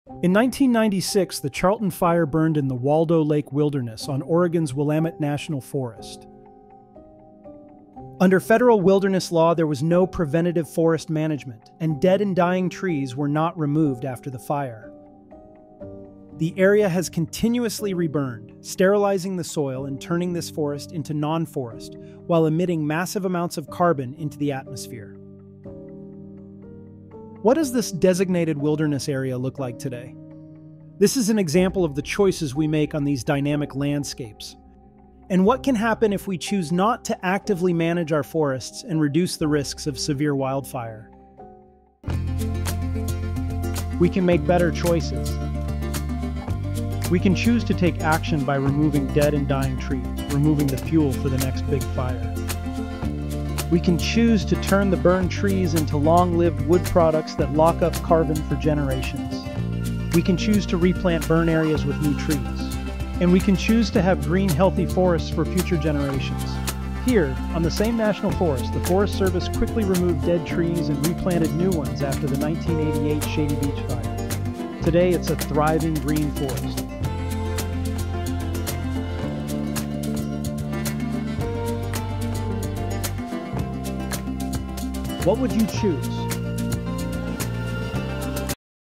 This is What Happens When Wildfires Burn Again and Again
When wildfires strike our national forests, what happens next determines their future.

In Oregon’s Willamette National Forest, the 1996 Charlton Fire burned inside a designated Wilderness area where no trees could be salvaged or replanted. Decades later, it continues to reburn, destroying soil, releasing carbon, and turning forestland into wasteland.

Just miles away, the Shady Beach Fire burned in 1988. There, foresters removed dead trees and replanted a new forest. Today it’s thriving, green, and alive. This is the difference between action and inaction.

We can choose to remove dead trees, reduce fuels, and replant new forests, so future generations inherit healthy, green landscapes instead of ashes. What do you choose?